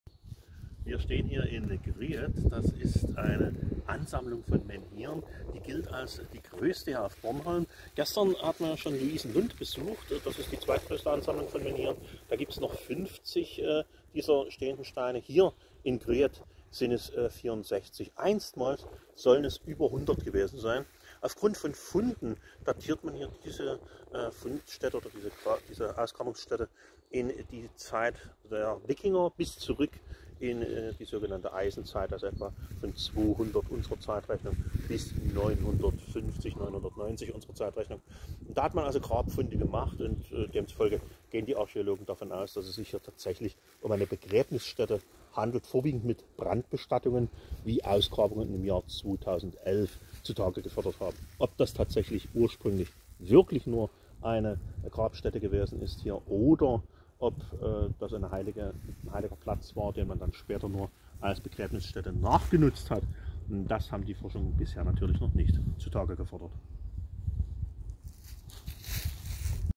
Die Menhire von Gryet
Das Menhirefeld von Gryet ist das größte in Bornholm. Einst standen hier wohl mehr als 100 der aufrecht stehenden Steine. Davon sind heute noch 64 erhalten.